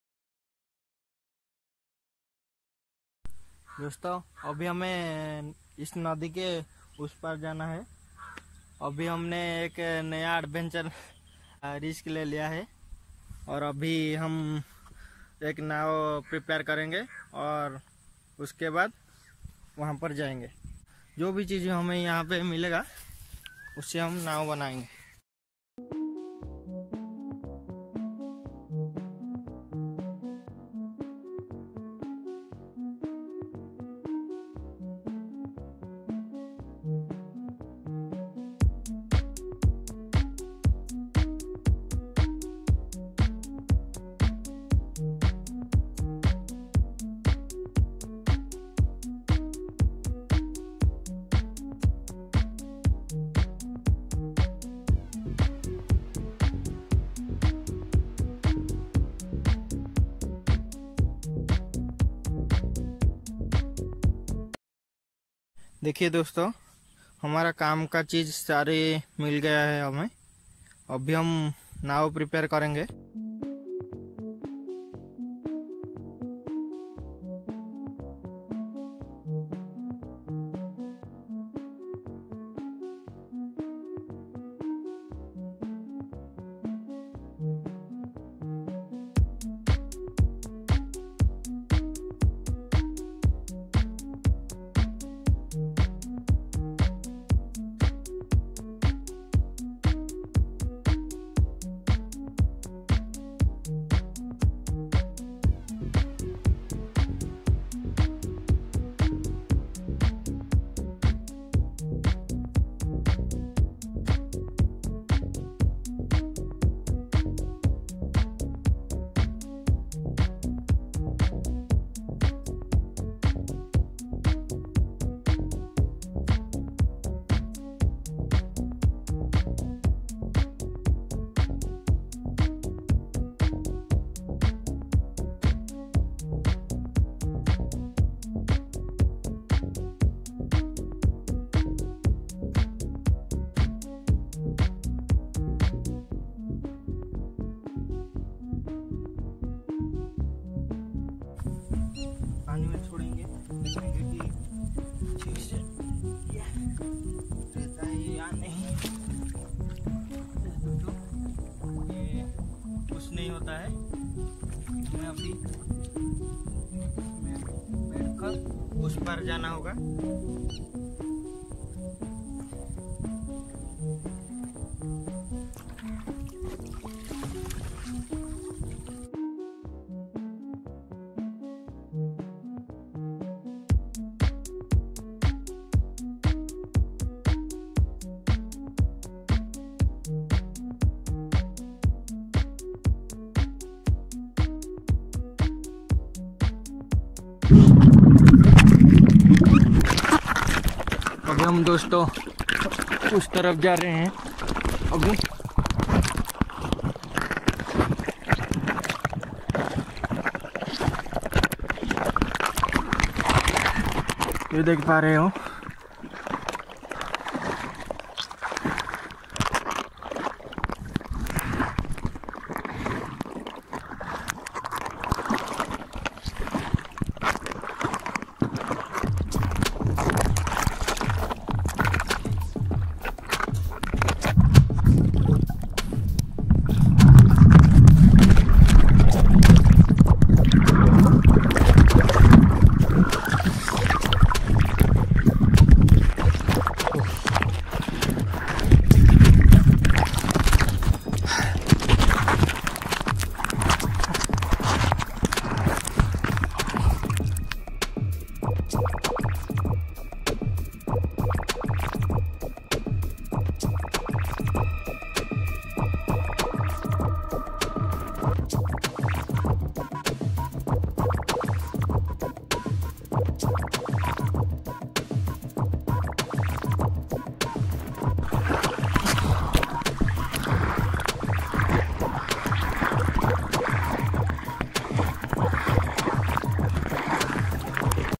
Man vs Wild in hindi episode:1
Crossing the road by Water At Mahanadi which is the biggest river on the Odisha.
 continue to next videos